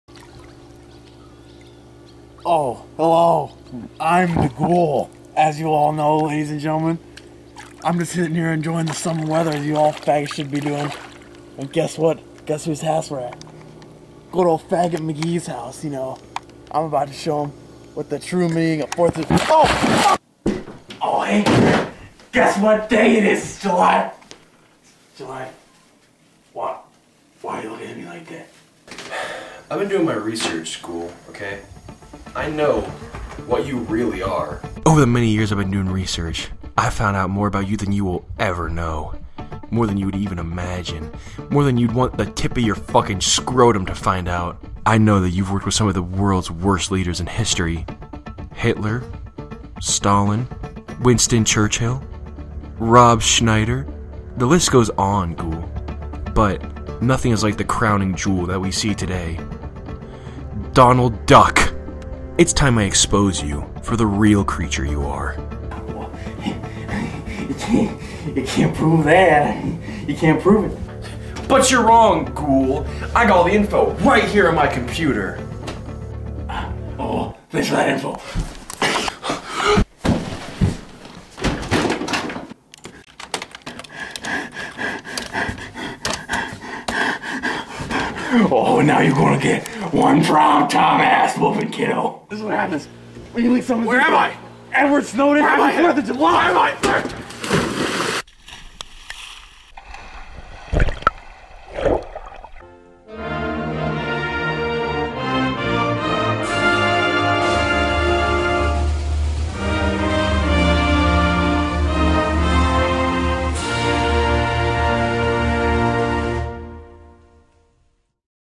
4th of Ghouly
The ghoul has come to teach Garrett to feel pride for his country, but what he doesn't know is that Garrett has found out his dark secrets...